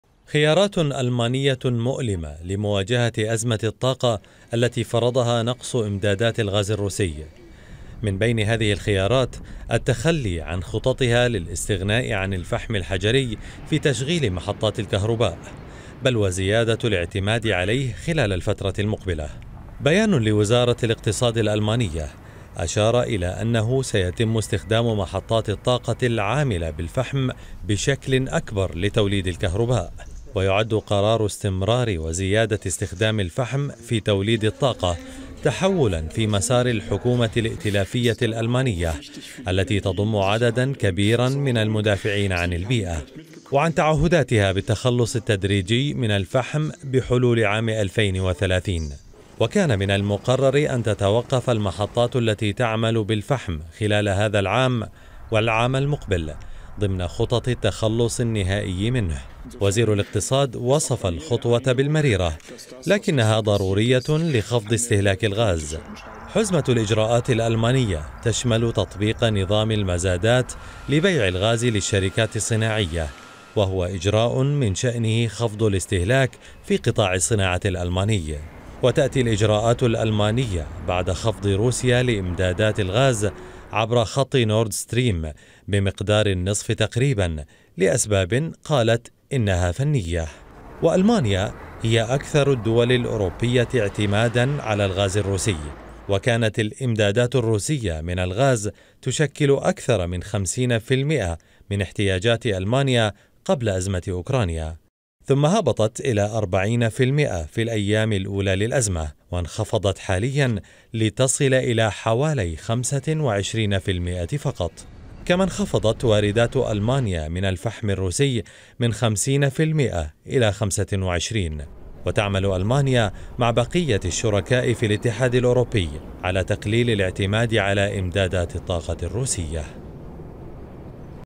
أزمة أوكرانيا..ألمانيا تلجأ للفحم الحجري لمواجهة نقص الغاز الروسي
أعلنت ألمانيا عن إجراءاتٍ طارئة للتعامل مع نقص إمدادات الغاز الروسي.
وتشمل الإجراءاتُ الاستمرارَ والتوسُّع في استخدام الفحم في محطات توليد الطاقة.
ويُعد هذا التوجه تراجعا عن خطط  ألمانيا للتخلص نهائيا من استخدام الفحم الحجري.